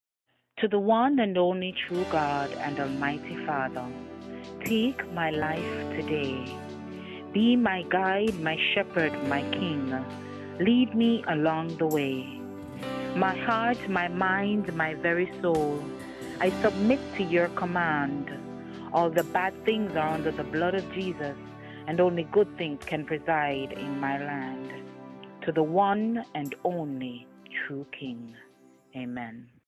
The Poet's Prayer: To the one and only true King
Revealing the Christian Life with Poetess Denise Fyffe
It is my hope to continue sharing and gaining an understanding about the real walk with Jesus Christ.

Books available